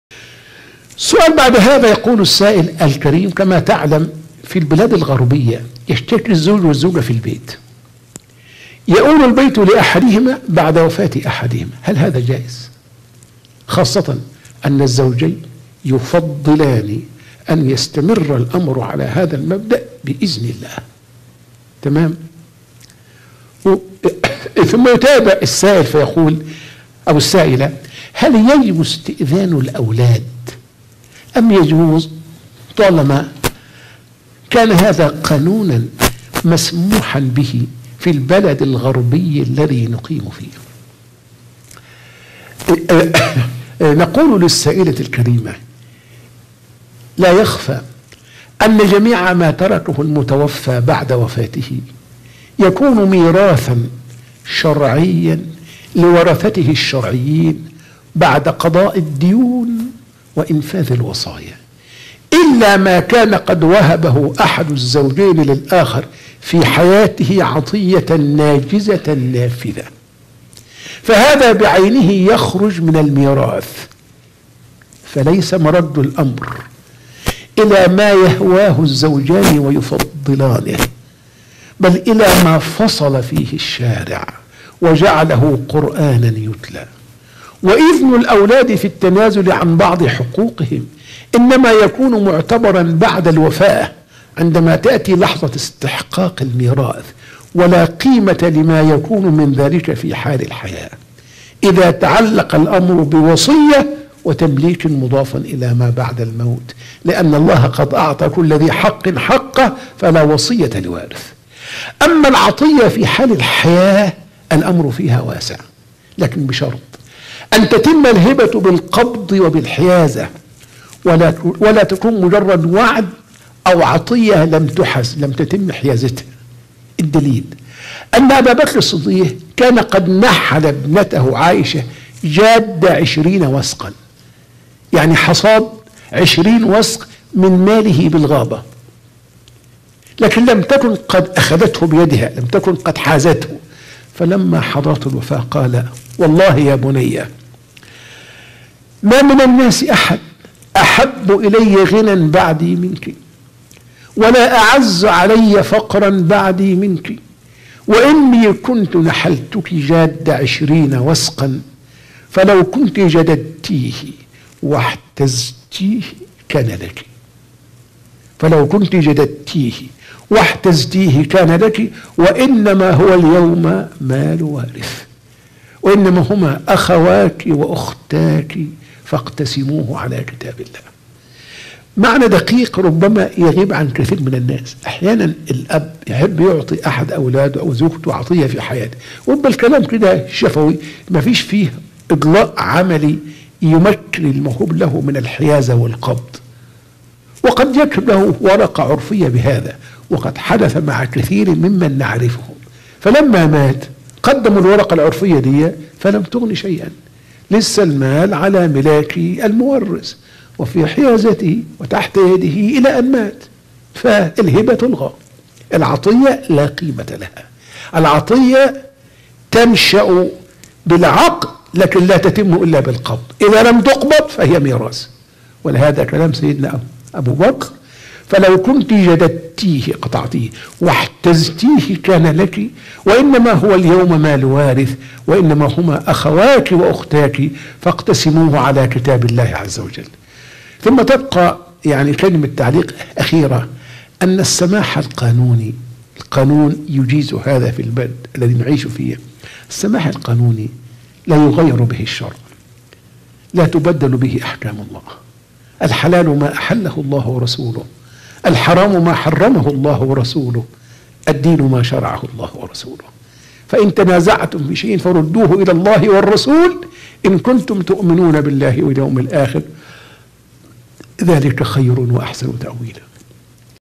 الأخذ بالقوانين الغربيه فى الوصايا و المواريث/فتاوى على الهواء مباشرة/الخميس(2021/2/4م)
الحمد لله، والصلاة والسلام على رسول الله، وعلى آله وصحبه ومن والاه، أما بعد: فإن جميع ما تركه الميت بعد وفاته يكون ميراثا شرعيا لورثة الشرعيين، بعد قضاء الديون وإنفاذ الوصايا،

إلا ما كان قد وهبه أحد الزوجين للأخر في حياته عطية ناجزة نافذة فهذا بعينه يخرج من الميراث،

فليس مرد الامر إلى ما يهواه الزوجان ويفضلانه، بل إلى ما فصل فيه الشارع، وجعل قرآنا يتلى!

وإذن الأولاد في التنازل عن بعض حقوقهم يكون معتبرا بعد الوفاة واستحقاق الميراث، ولا قيمة لما يكون من ذلك في حال الحياة إذا تعلق الأمر بوصية وتمليك مضاف إلى ما بعد الموت.
أما العطية في حال الحياة فالأمر فيها واسع، على أن تتم الهبة بالقبض والحيازة، ولا تكون مجرد وعد أو عطية لم تحز،
لما صح أنَّ أبا بكرٍ الصِّديقَ كان نحَلَ ابنته عائشة جادَّ عشرين وَسقًا من مالِه بالغابةِ، فلما حضرتْه الوفاةُ قال: واللهِ يا بُنيَّةُ ما من الناسِ أحدٌ أحبُّ إليَّ غنًى بعدي منكِ، ولا أعزُّ عليَّ فقرًا بعدي منكِ، وإني كنتُ نحَلتُكِ جادَّ عشرين وَسقًا، فلو كنتُ جدَدتِيه واحتزتِيه كان لكِ، وإنما هو اليومَ مالُ وارثٍ، وإنما هما أخواك وأختاك فاقتسِموه على كتابِ اللهِ،
والسماح القانوني يا صاحبة الفضيلة لا يغير به الشرع، ولا تبدل به الاحكام، فالحلال ما أحله الله ورسوله، والحرام ما حرمه الله ورسوله، والدين شرعه الله ورسوله،
والله تعالى أعلى وأعلم